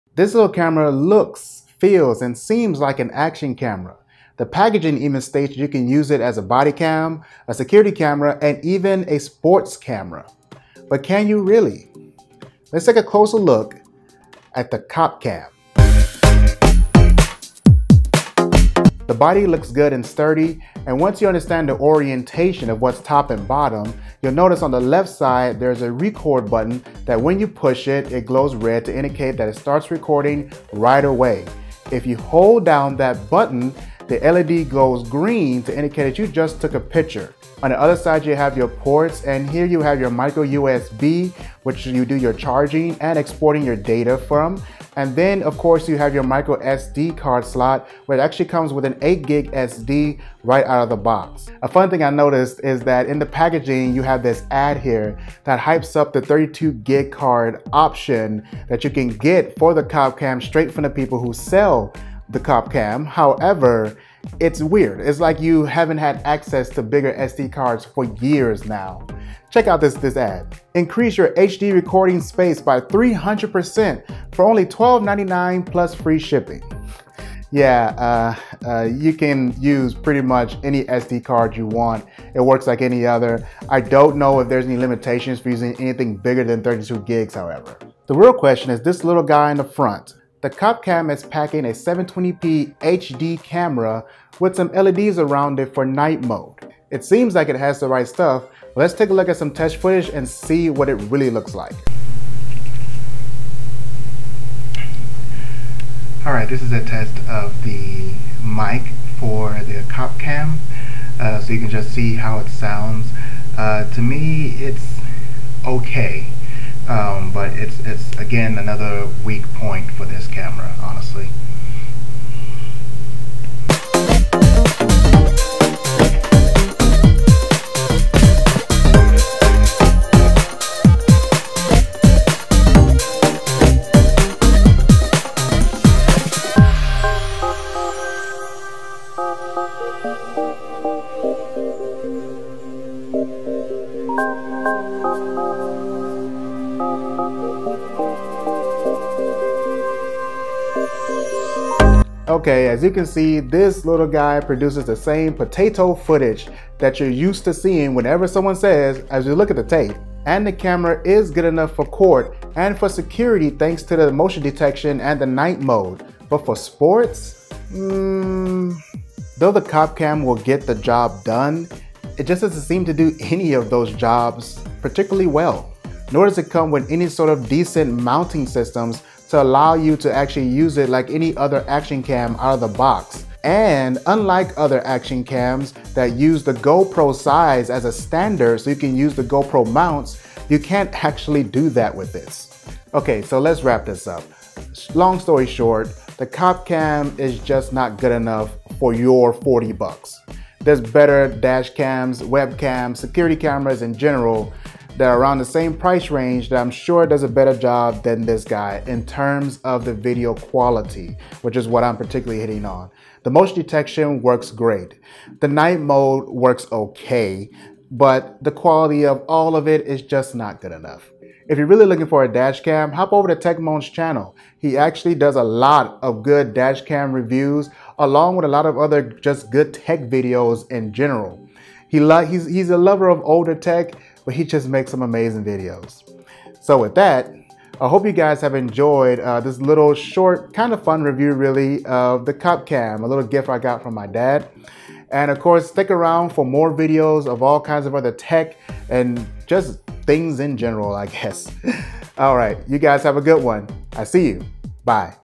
Cop Cam - Security Camera - Great Budget Tech or Bust!?
My dad snagged this little security cam up and tossed it over to me to take a look. Let's have a quick look at the Cop Cam!

Better Security Cameras -
(Amazon US)
(Amazon UK)

Gear List